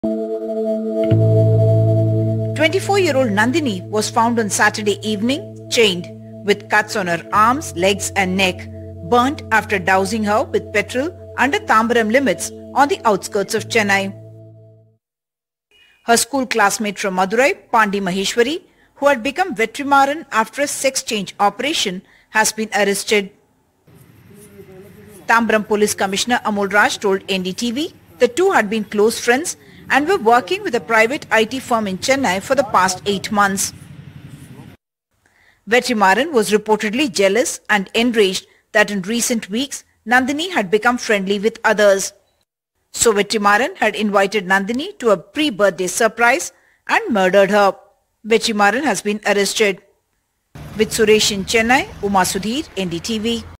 Woman Techie Burnt Alive By Classmate Who Underwent Sex Change To Marry Her
On the eve of her birthday, a 24-year-old software engineer in Tamil Nadu was chained, slashed with a blade and burnt alive by her former classmate who had undergone a sex change operation to marry her, police said. The disturbing incident unfolded in Thalambur near Kelambakkam, a quiet southern suburb of Chennai. According to police reports, 26-year-old Vetrimaran alias Pandi Maheswari, under the guise of a birthday surprise, blindfolded 24-year-old R Nandhini and subjected her to an unimaginable ordeal involving chains and petrol.